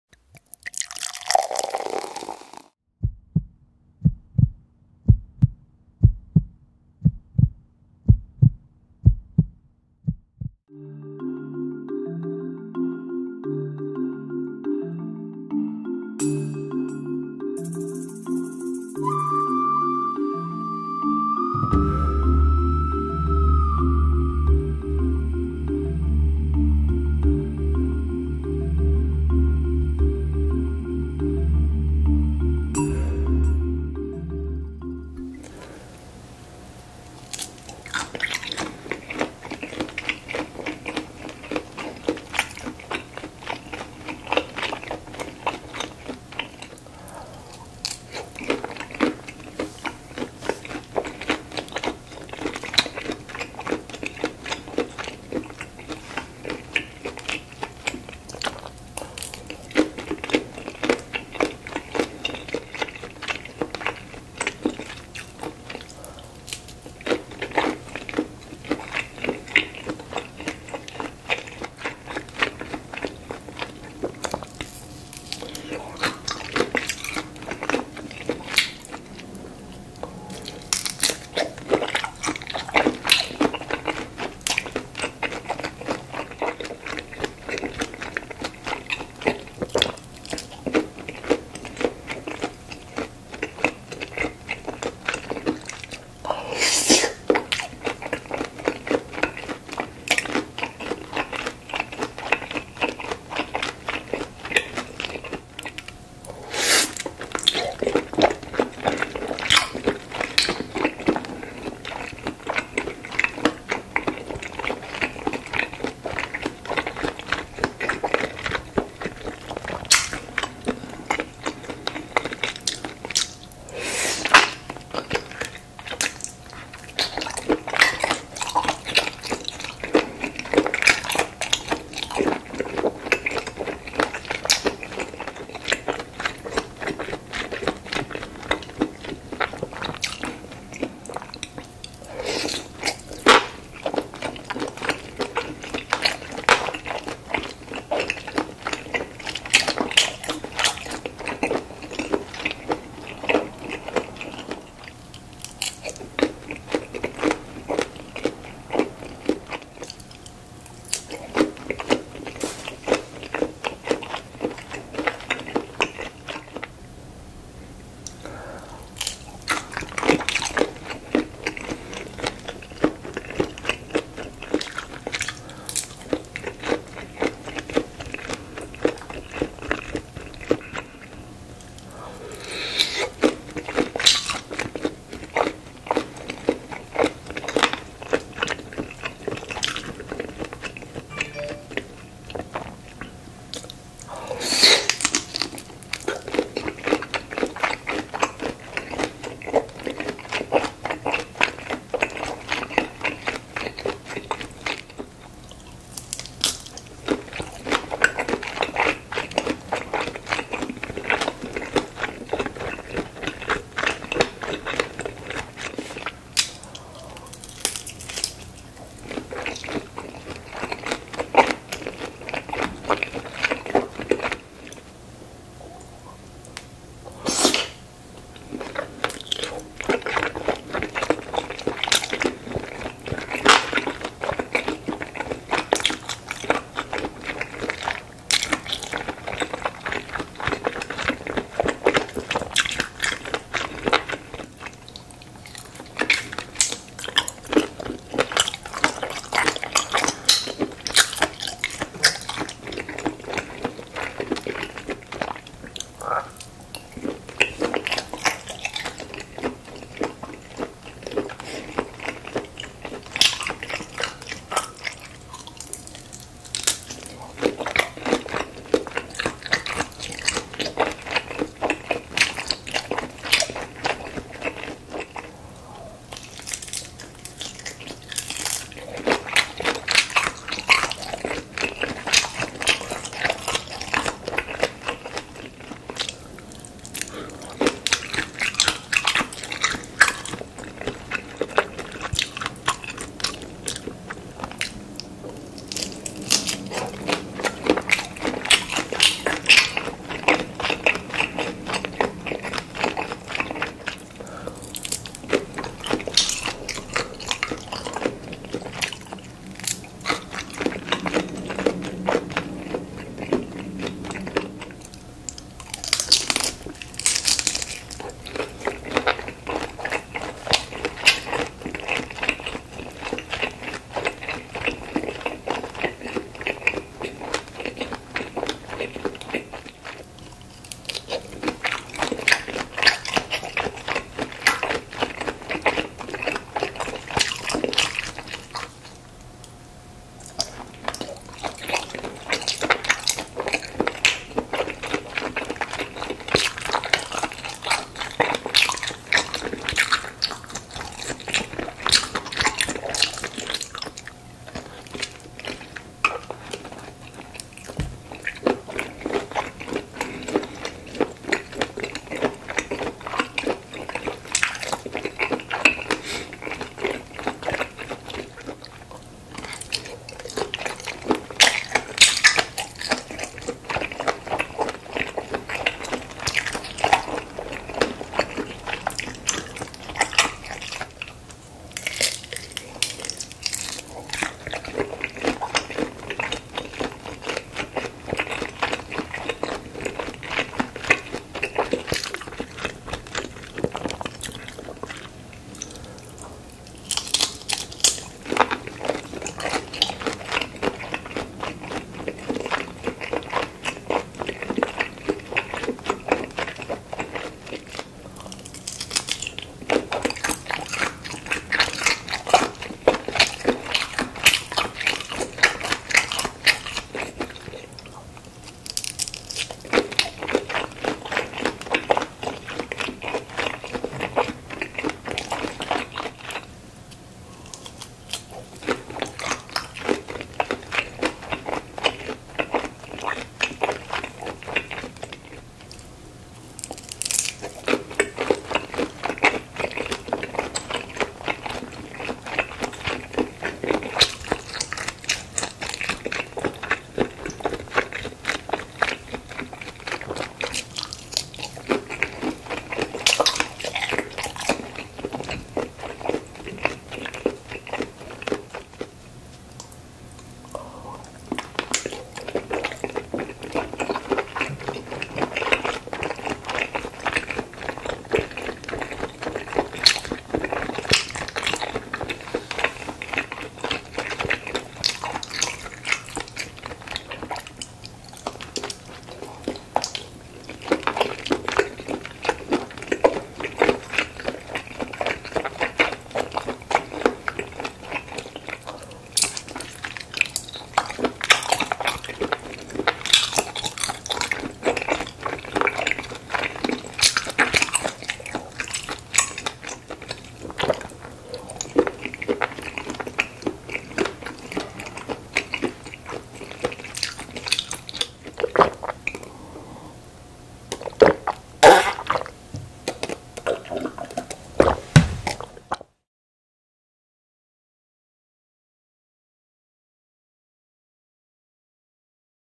(ASMR) SHRIMP COCKTAIL & OCTOPUS
ASMR

MUKBANG- Shrimp Cocktail & Octopus

All food from
Costco

Showing weekly cheat meals and healthy meals